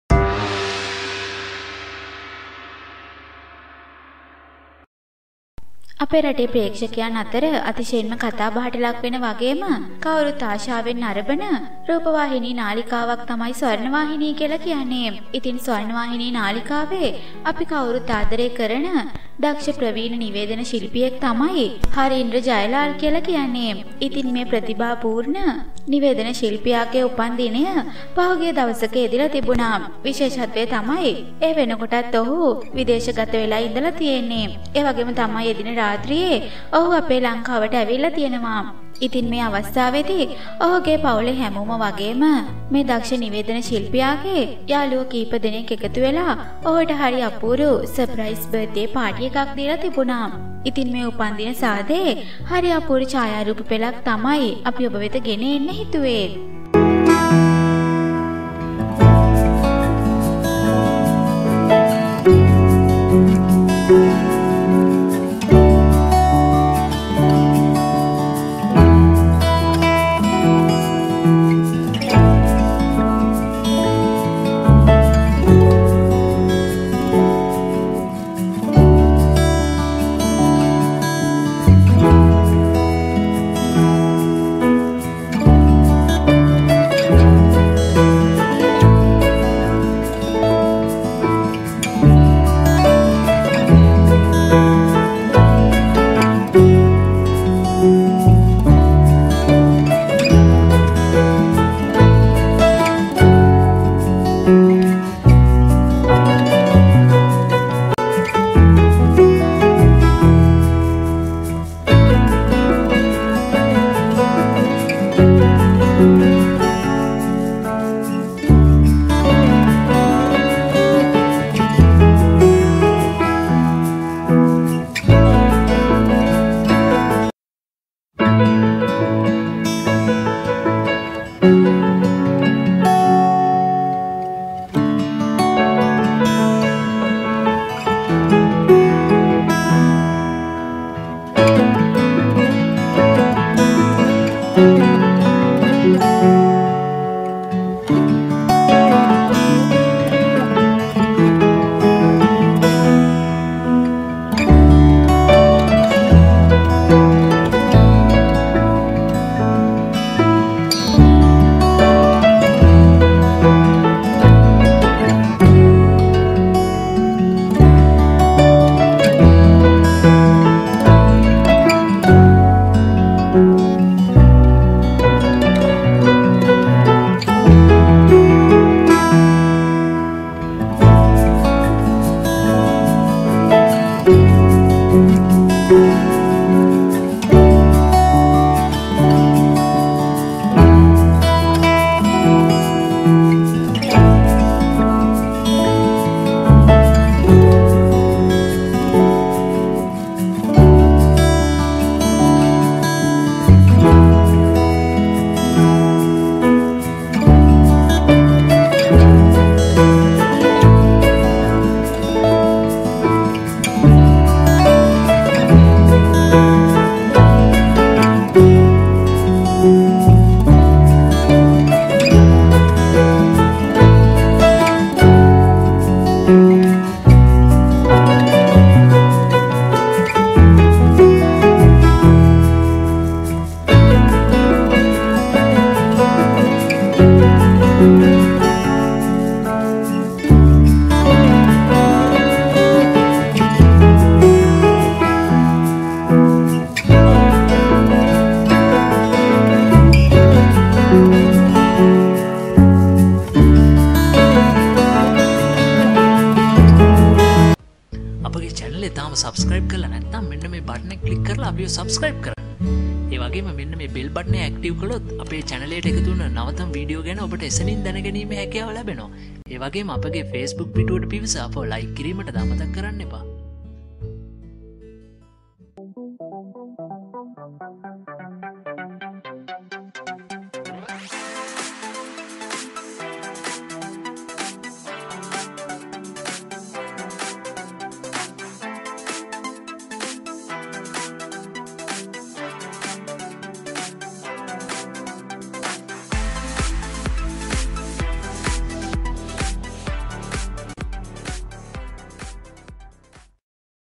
ස්වර්ණවාහිනි නිවේදක හරින්ද්‍ර ජයලාල්ගේ උපන්දිනයට ලැබුන සර්ප්‍රයිස් පාටිය මෙන්න Harindra Jayalal
ස්වර්ණවාහිනි නිවේදක හරින්ද්‍ර ජයලාල්ගේ උපන්දිනයට පවුලේ කට්ටියයි ළගම යාළුවොයි එකතුවෙලා දීපු සර්ප්‍රයිස් Birthday පාටි එක Harindra Jayalal